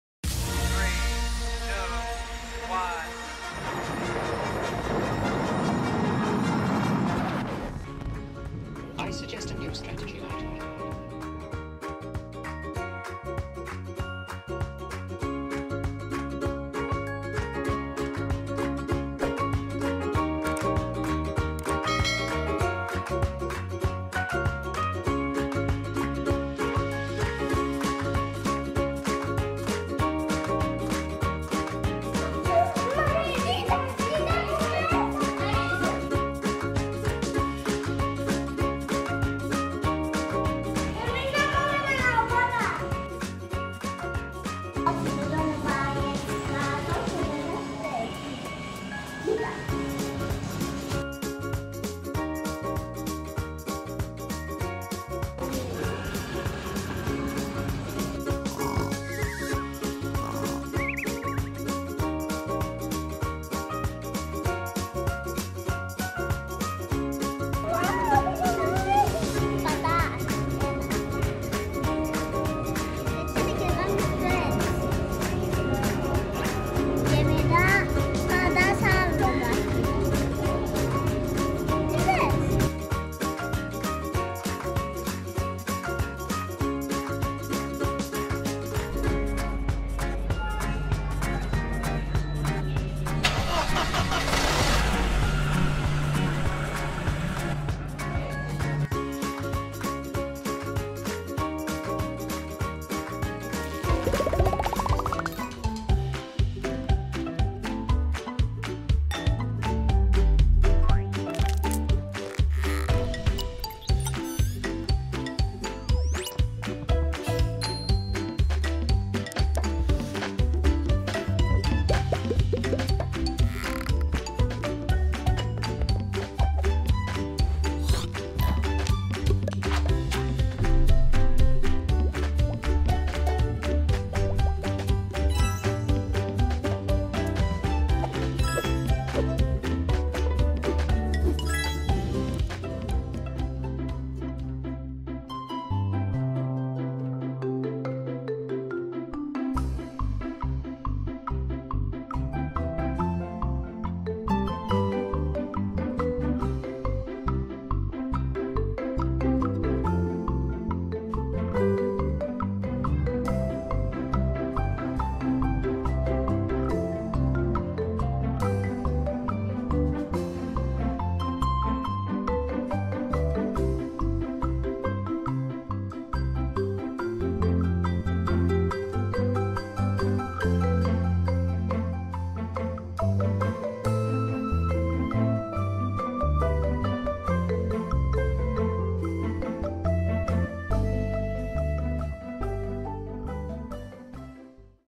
Scienceworks Melbourne | Η ζωή στο διάστημα | Life in Space
Scienceworks Melbourne
We explore Melbourne's Science Museum. 🚀